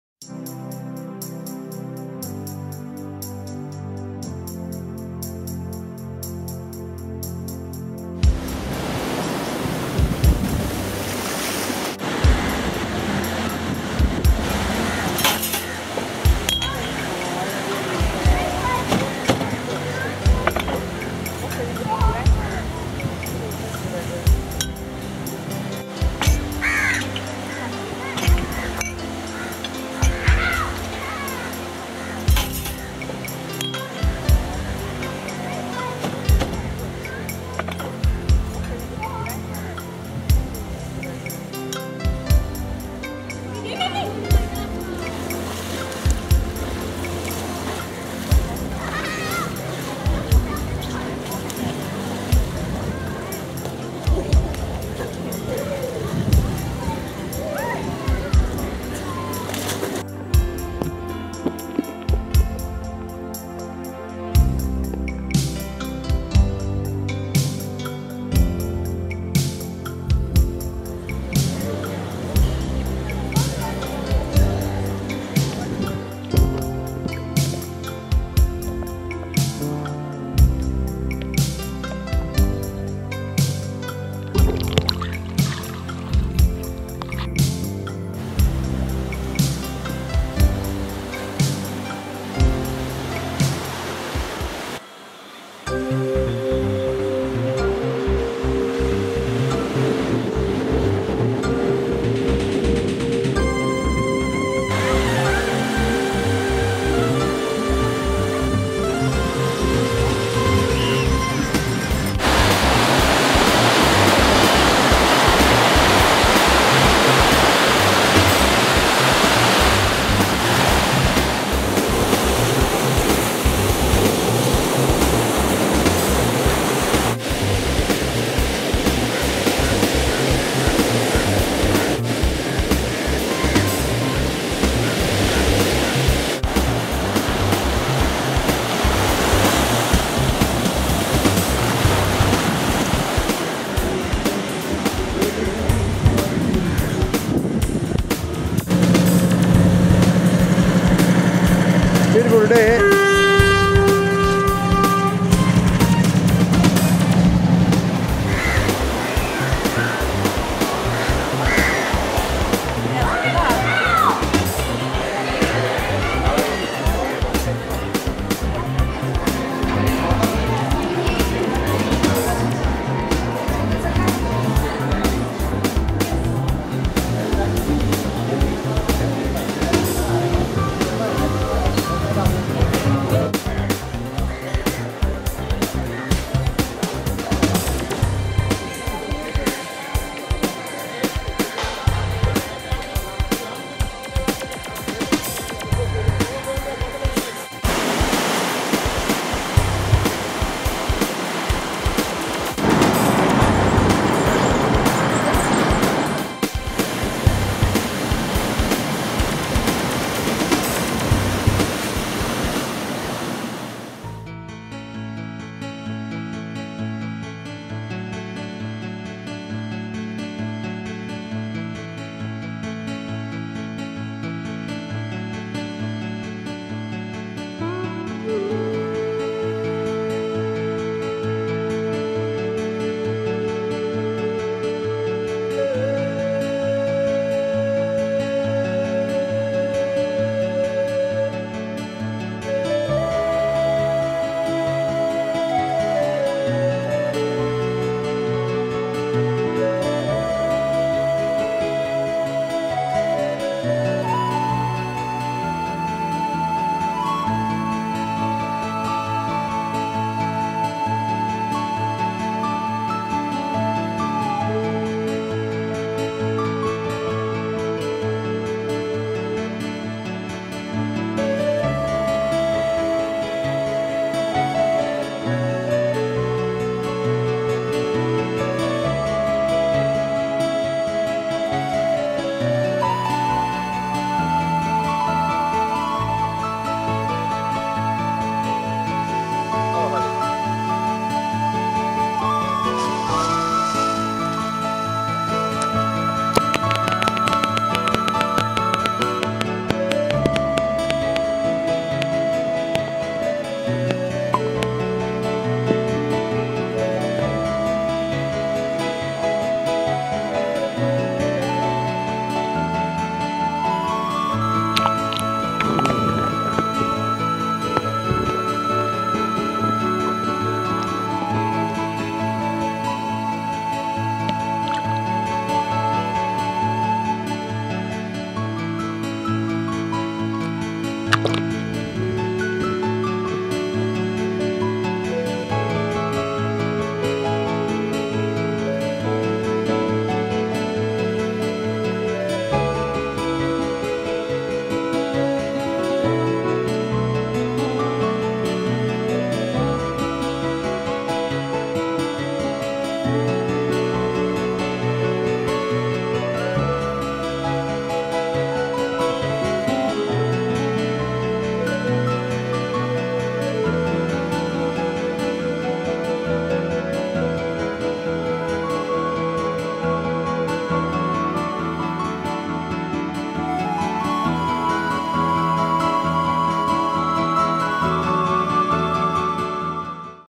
Sri Lanka - Swimming
Swimming in Sea and pools with underwater shots! Beautiful Mt. Lavinia beach, Mount Lavinia Hotel and  Club Palm Bay Hotel Maravila, Sri Lanka. Breathtaking Sunsets !